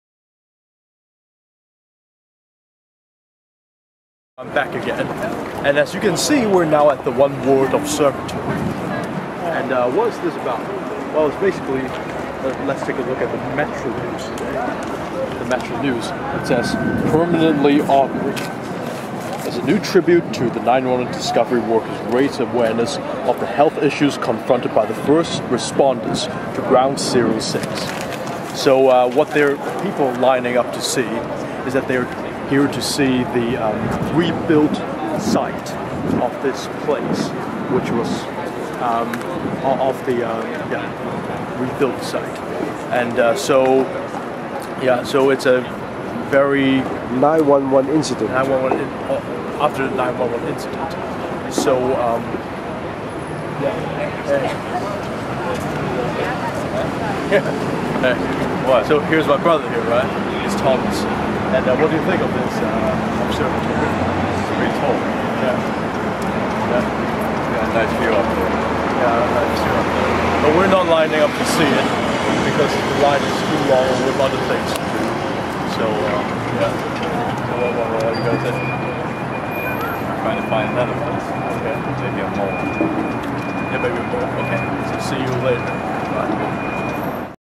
Learning at the One World Observatory
In lower Manhattan, we come across the One World Observatory and see a line forming waiting for their turn to see New York from high up.
Presenter: Oscar Fung and Thomas Fung
Producer: Thomas Fung